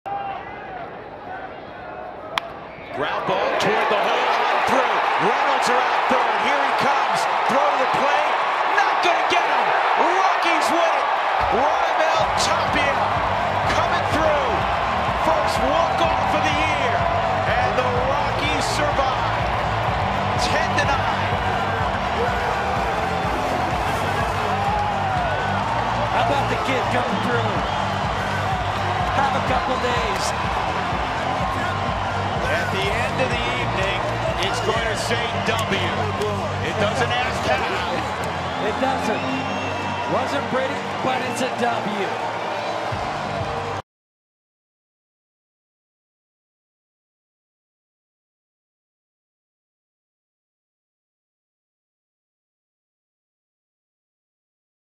Tapia wins it with walk-off single in the 9th
6/15/17: Raimel Tapia ends a wild game with a single through the right side that scores Mark Reynolds from second for a 10-9 walk-off win